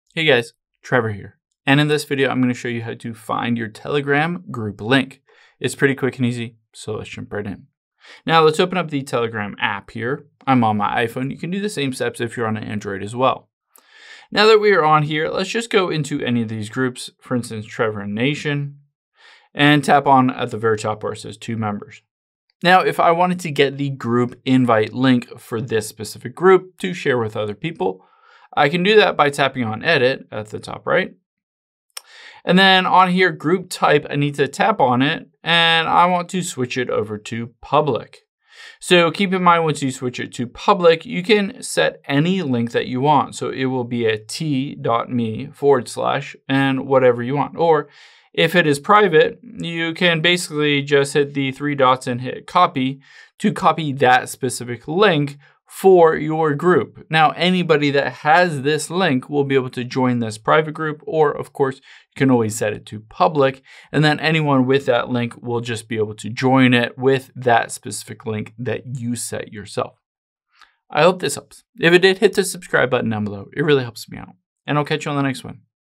How To Find Telegram Group Link
Here's how to find Telegram group links so you can see easy ways to share groups with other people so they can join easily.